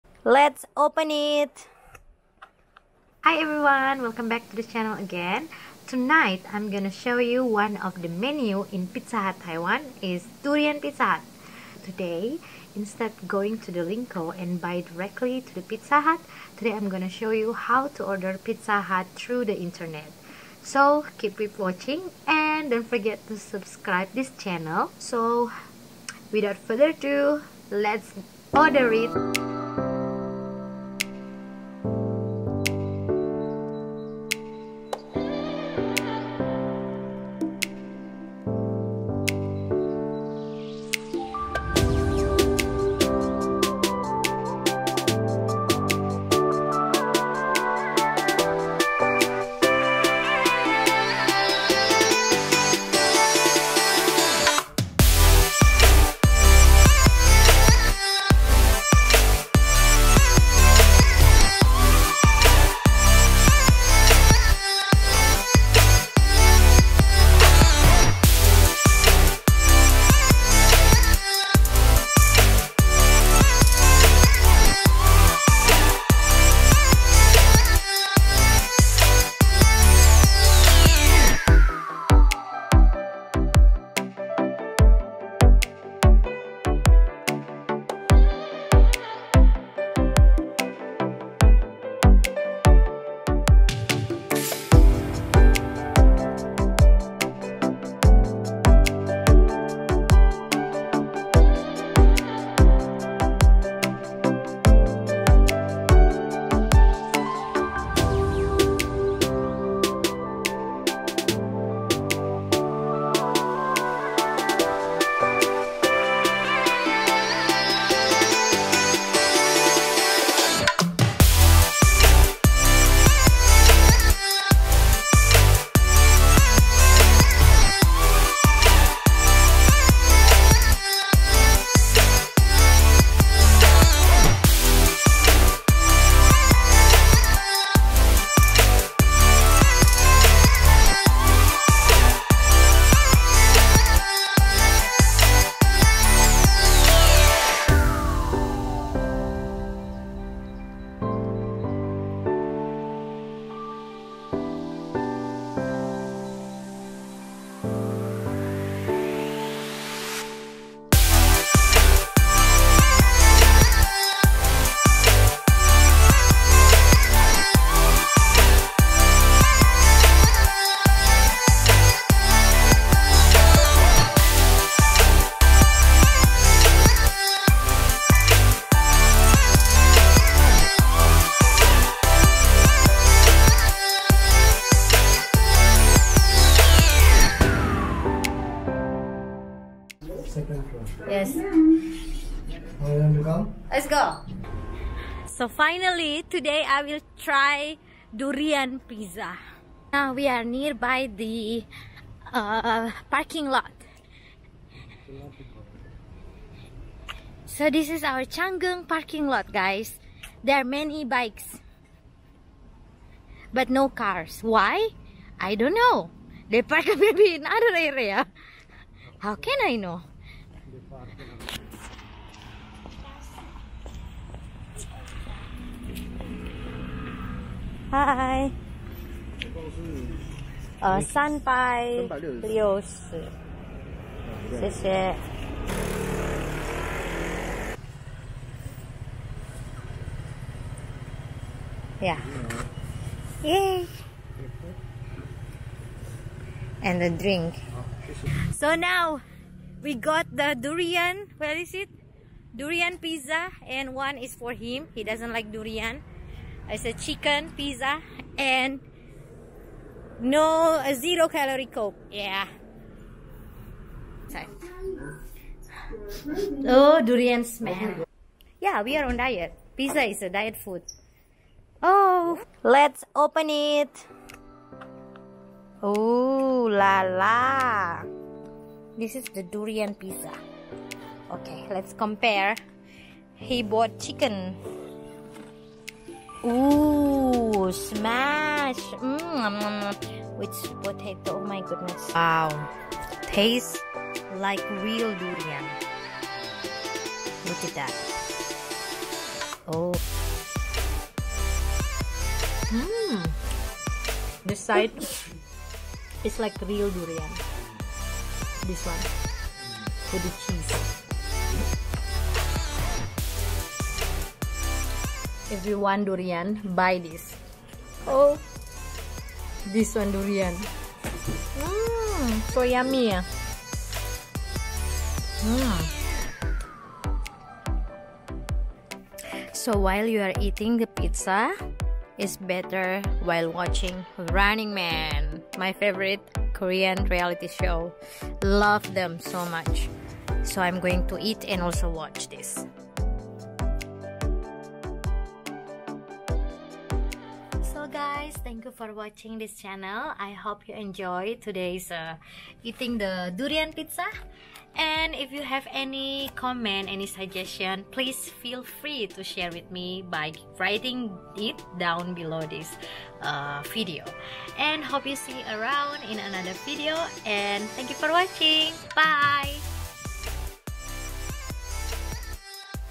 Pizza Hut 必勝客 Time During Project Time| PhD life in Taiwan
[English]
In this video, I want to show you guys how durian Pizza Hut looks like and how is the taste.
This video also provides a tutorial how to order Pizza Hut online.
Hopefully, it will be useful for you guys :D
[Bahasa]
Dalam video ini, saya ingin menunjukkan kepada kalian bagaimana penampilan Pizza Hut durian dan bagaimana rasanya.
Video ini juga menyediakan tutorial cara memesan Pizza Hut secara online.
Semoga bermanfaat bagi kalian: D